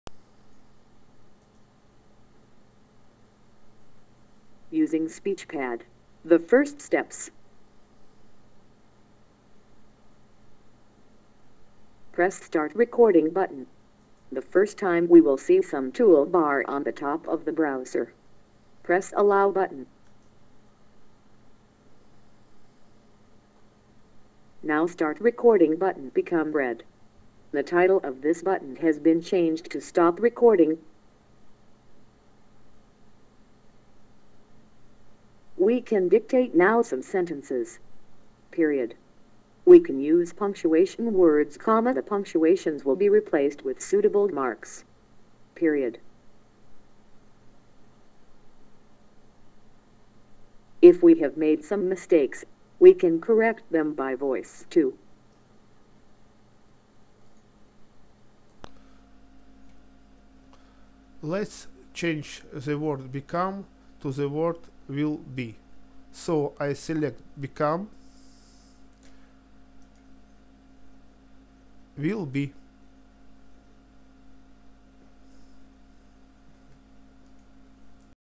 Speech Pad - online speech recognition service
The first steps of using Speech Pad for dictation.